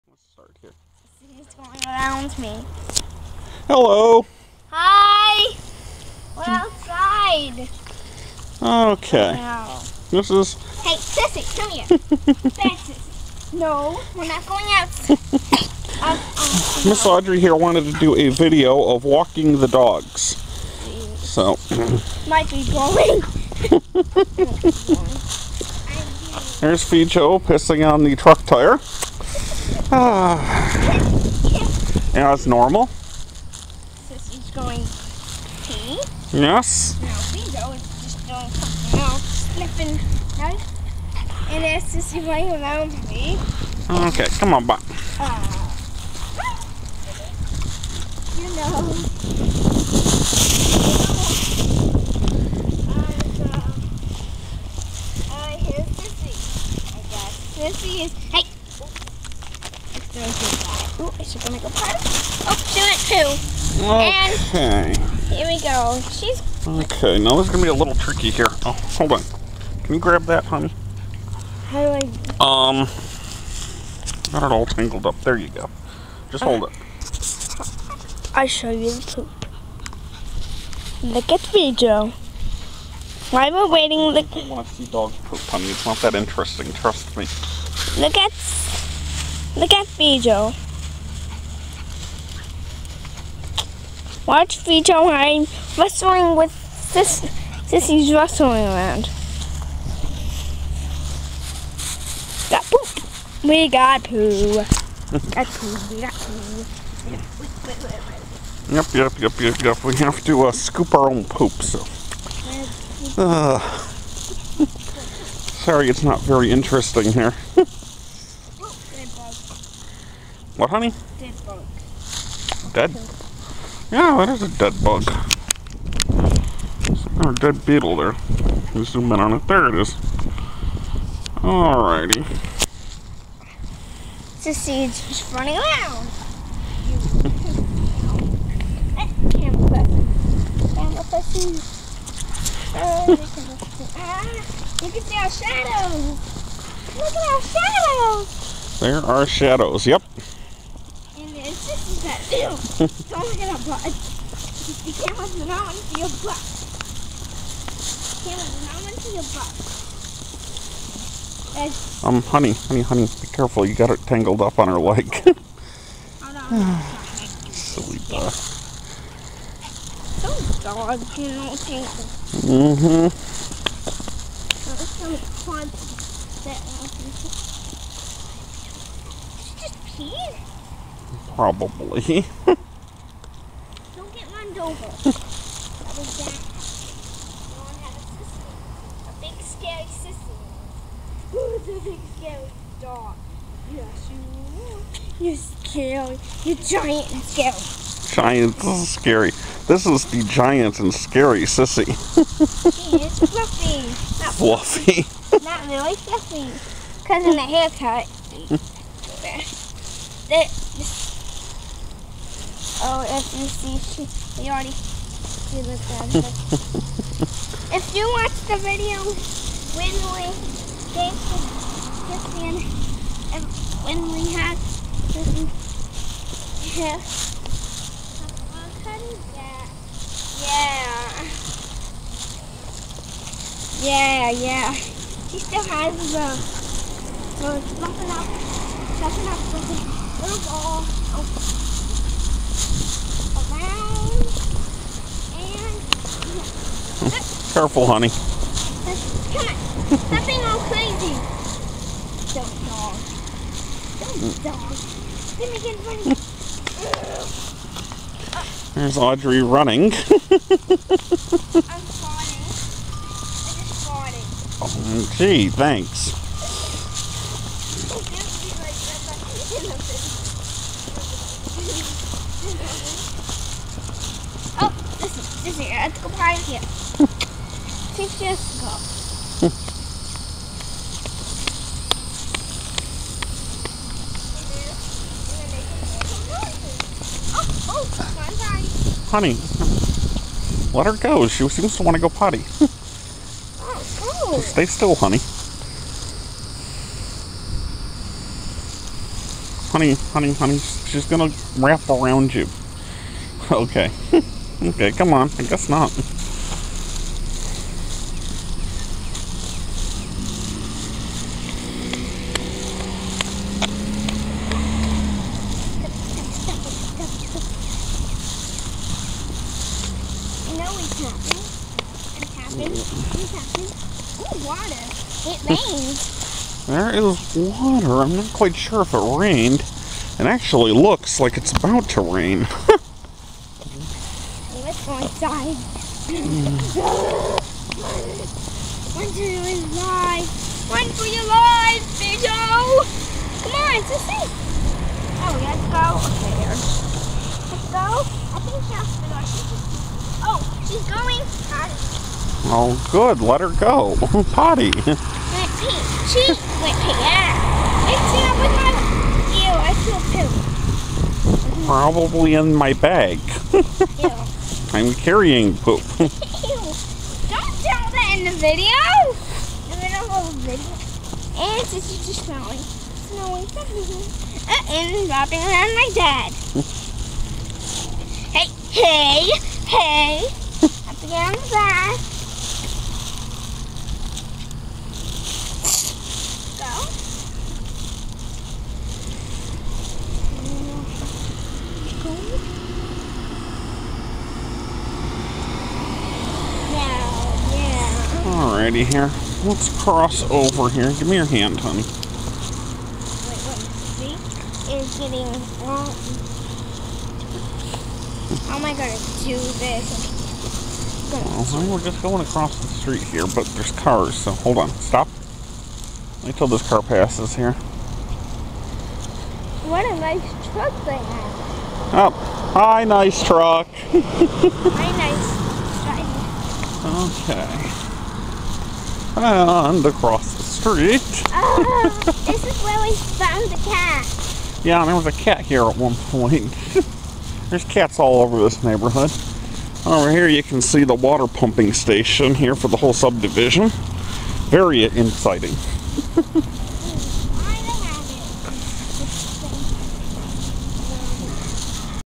Fun with walking the Scary Dogs
Audrey and me take our dogs for a walk. Sorry it is not longer, but the camera became full before half the walk.